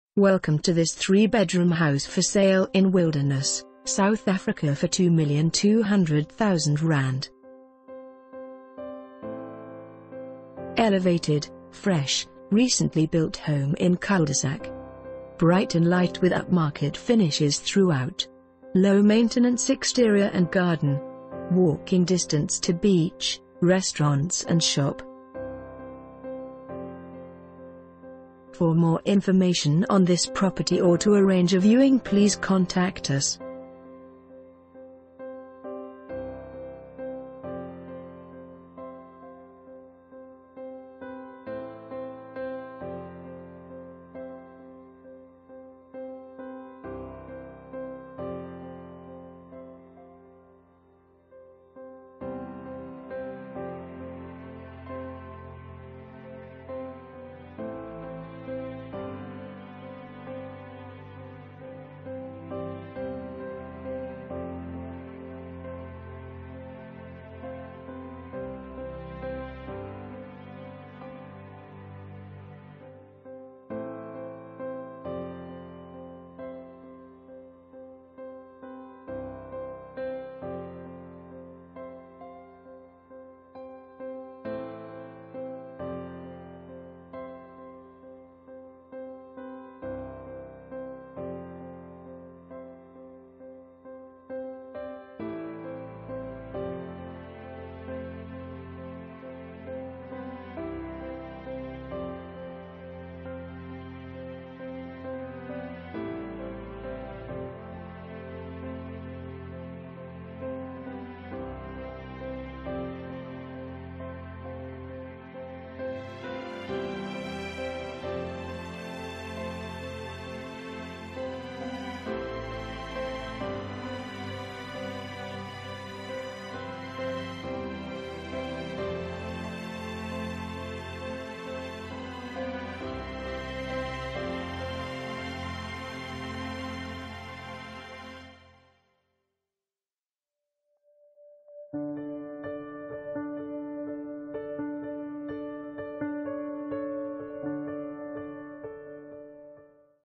3 Bedroom House For Sale in Wilderness, South Africa for ZAR 2,200,000...
Elevated, fresh, recently built home in cul-de-sac. Bright and light with upmarket finishes throughout. Low maintenance exterior and garden. Walking distance to beach, restaurants and shop.